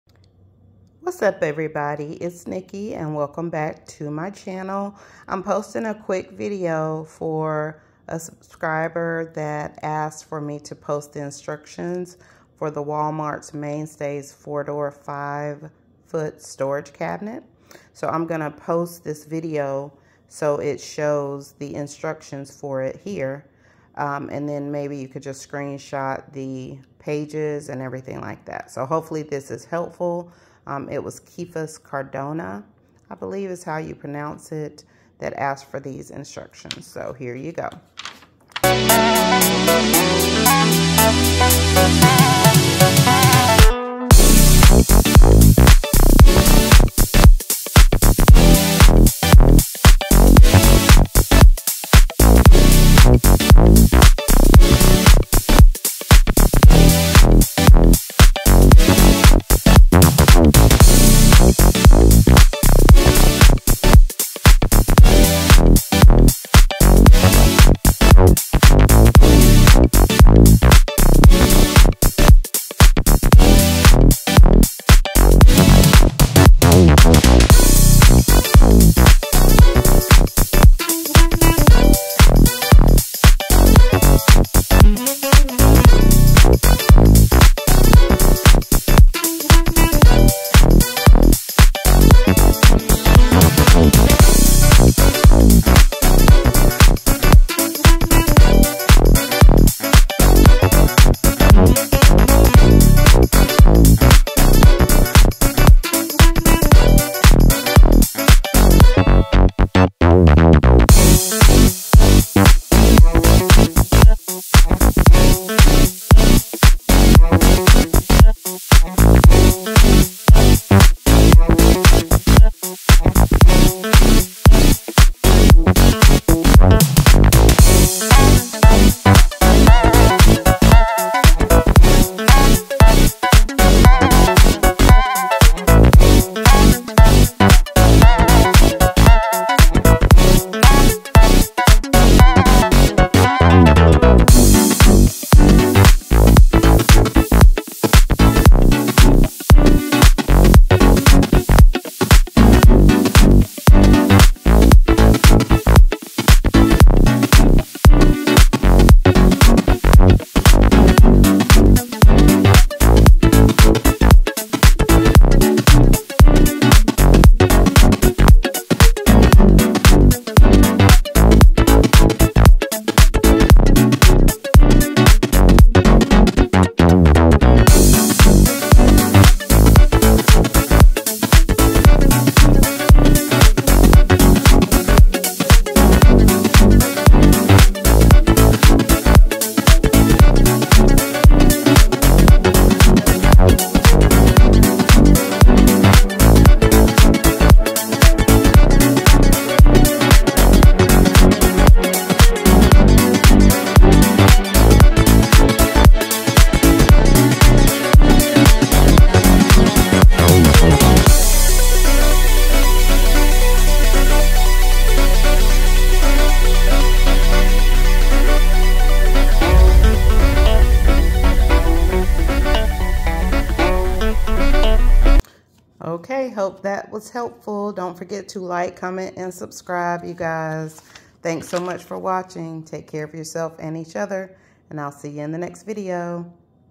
Assembly instructions for the Walmart Manstays 5' 4 door storage cabinet! 2024
INSTRUCTIONS FOR THE 4 DOOR 5' MAINSTAYS STORAGE CABINET AS REQUESTED BY A VIEWER.